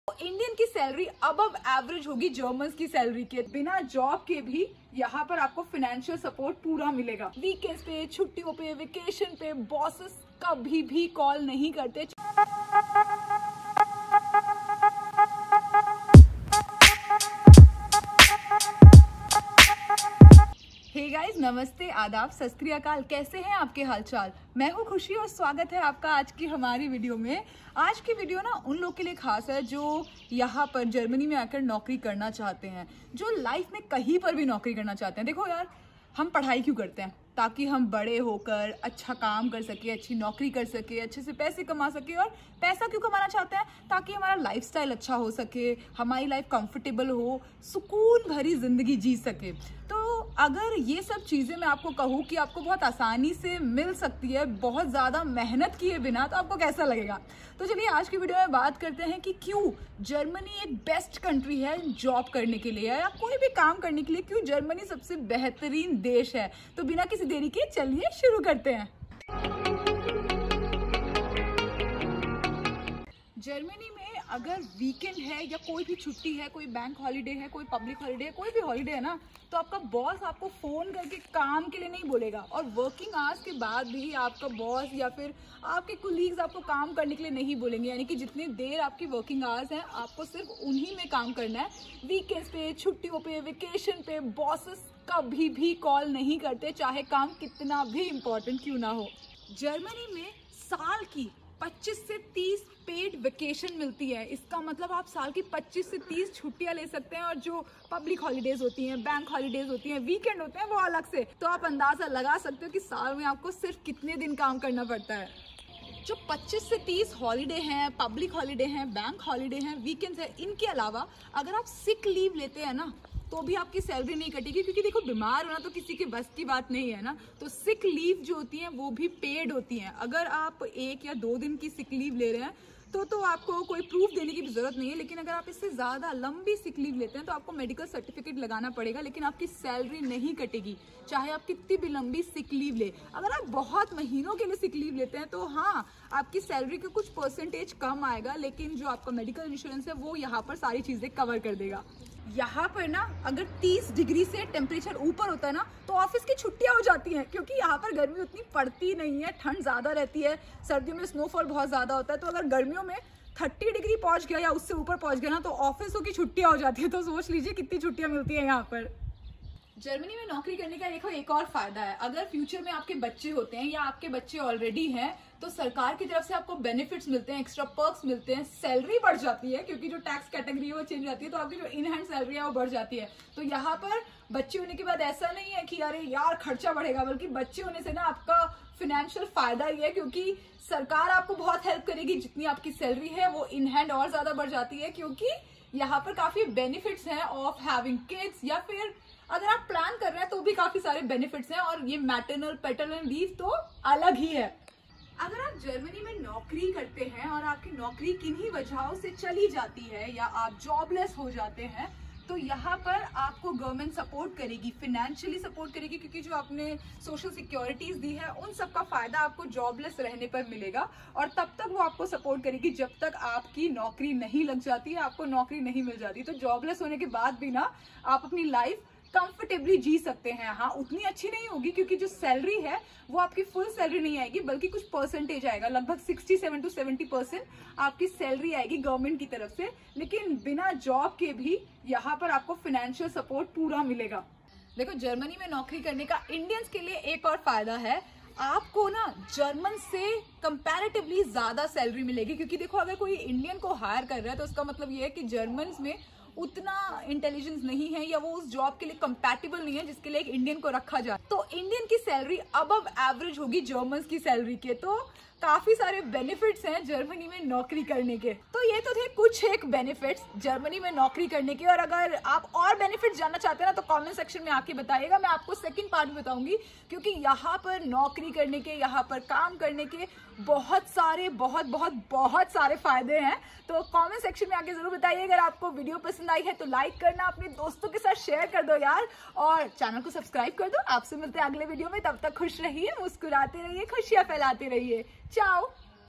Why Germany 🇩🇪 is the best Country to Work! | Desi Firangi
क्यों जर्मनी सबसे अच्छा देश है नौकरी करने के लिए?